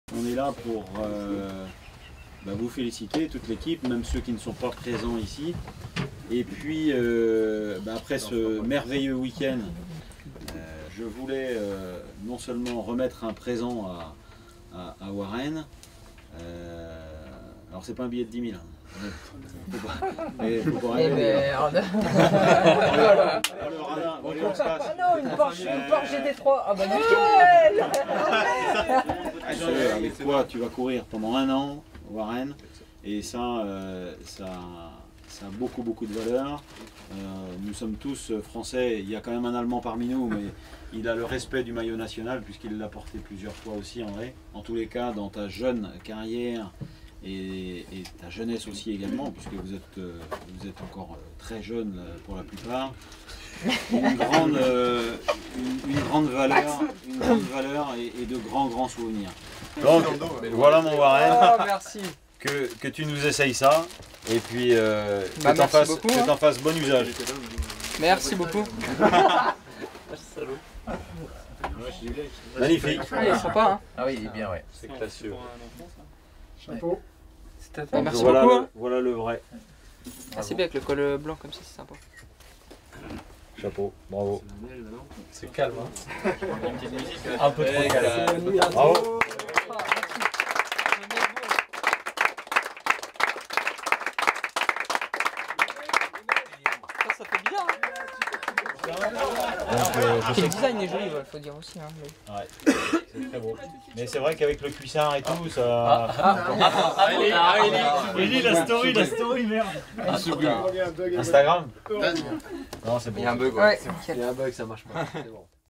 Warren Barguil a reçu son maillot de Champion de France !🇫🇷
📽️ " Ça fait bizarre quand même "

Vidéo : Elen Rius